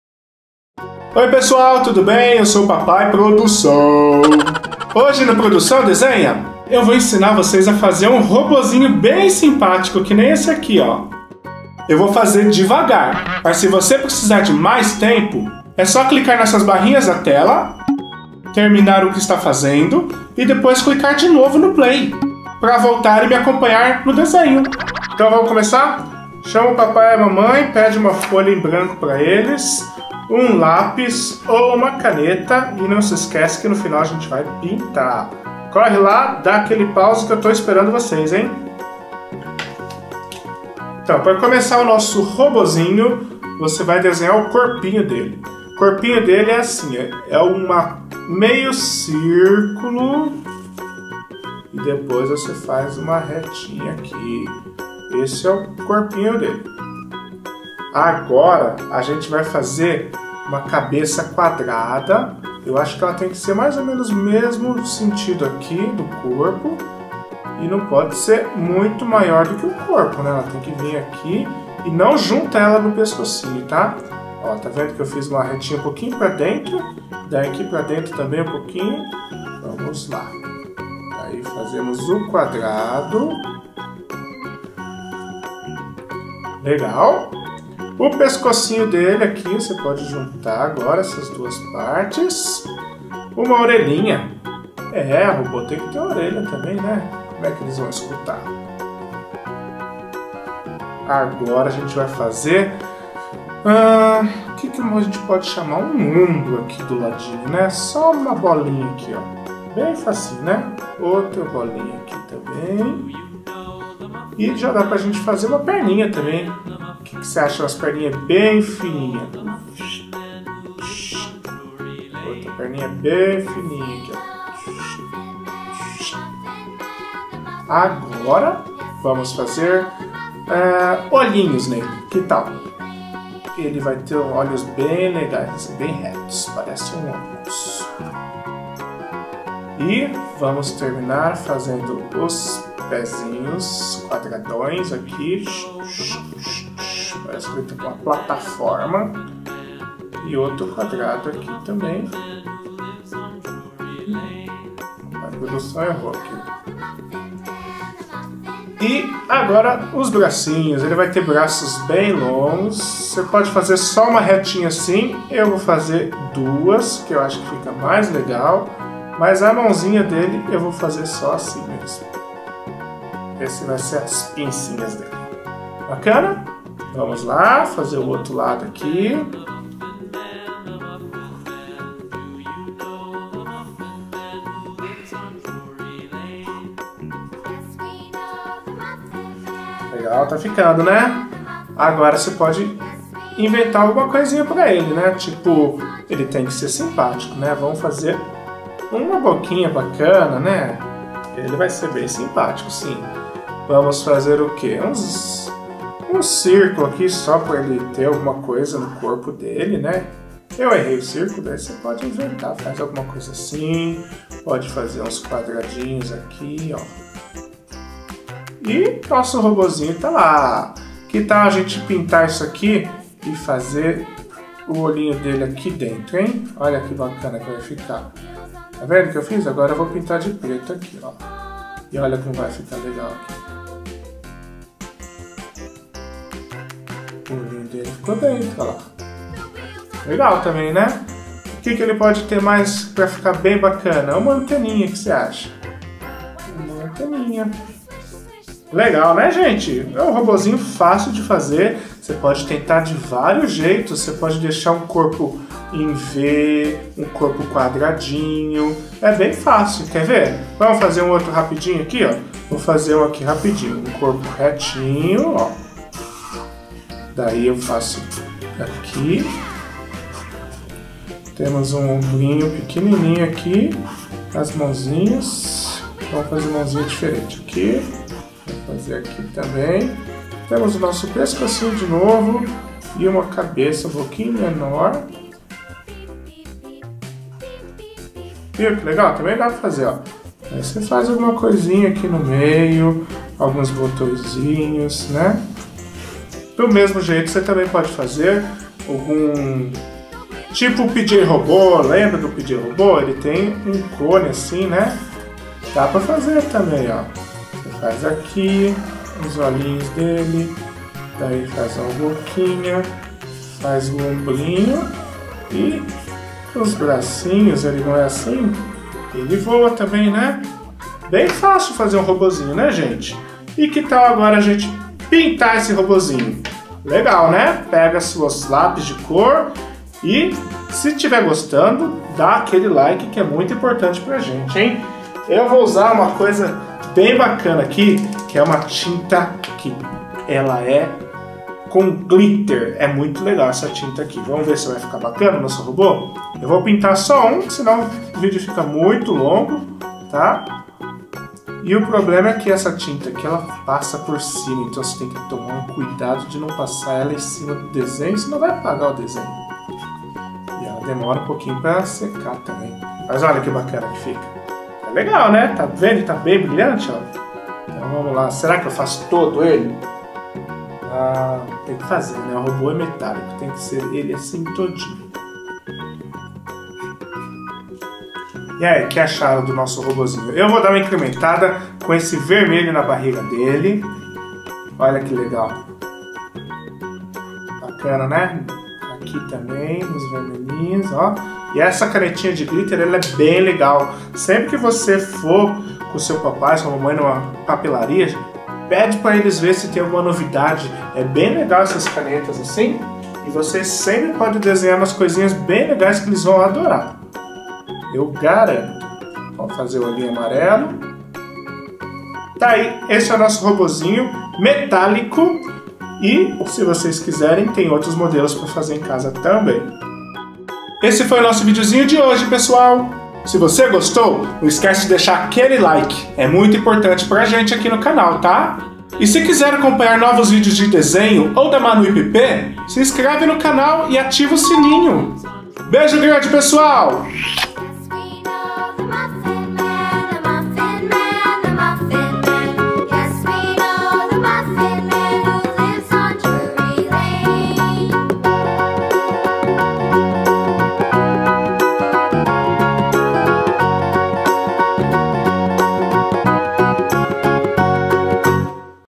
Papai Produção Desenha: um Robô
Papai Produção vai ensinar como desenhar vários robôs.
Vamos aprender?